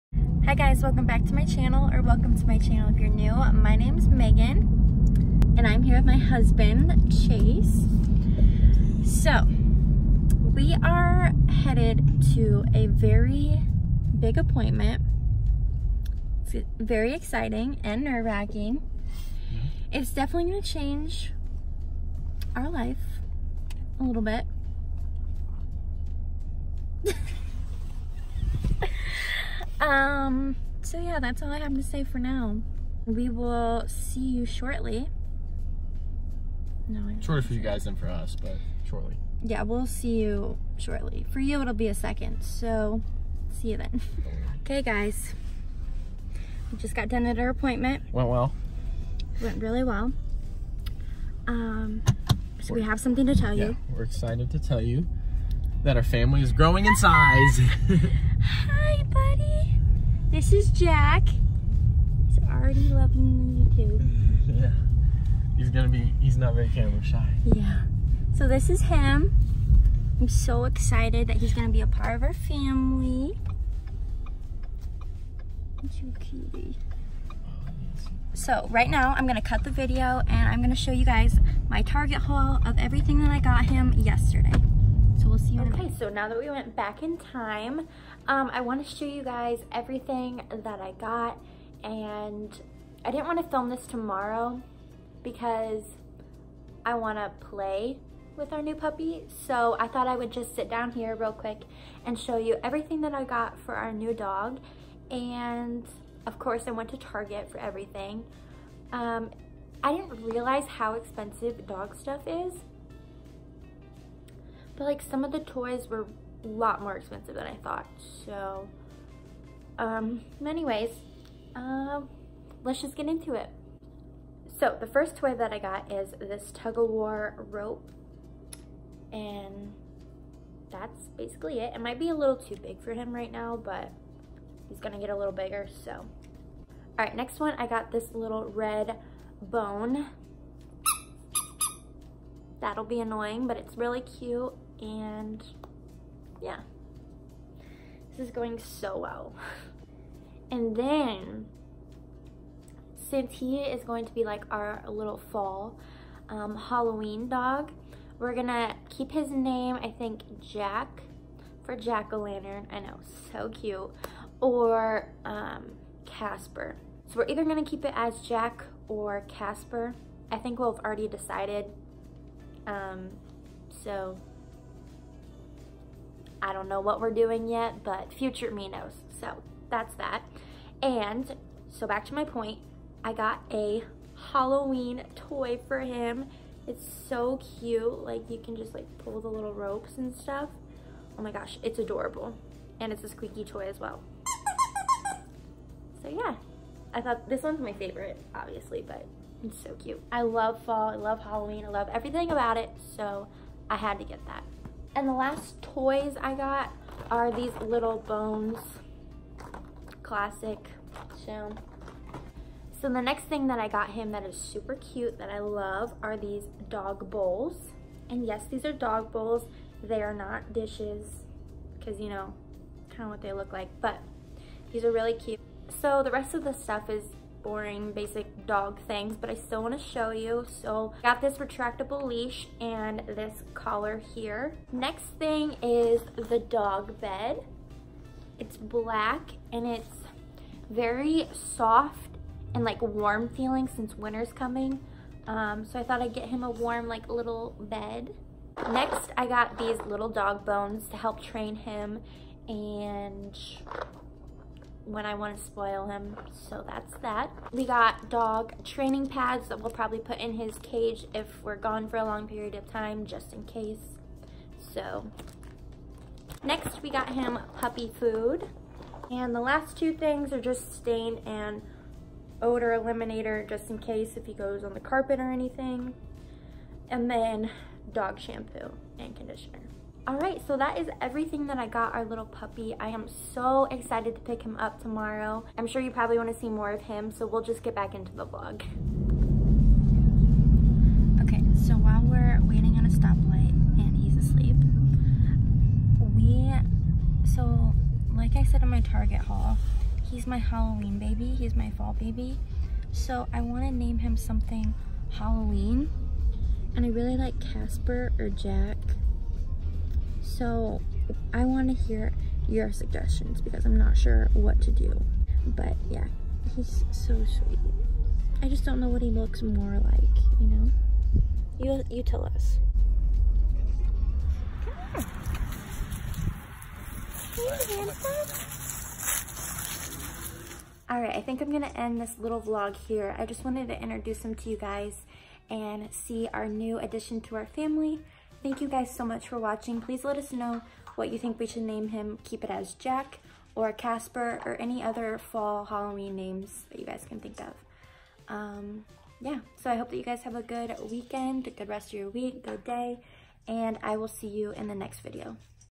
WE HAVE SOMETHING TO TELL YOU...
meet jack 💙
we adopted him from a foster home, where they rescued him and his mom and brothers and sisters from a high kill shelter in Kentucky.

he is the sweetest and we can’t wait to share his journey with you guys!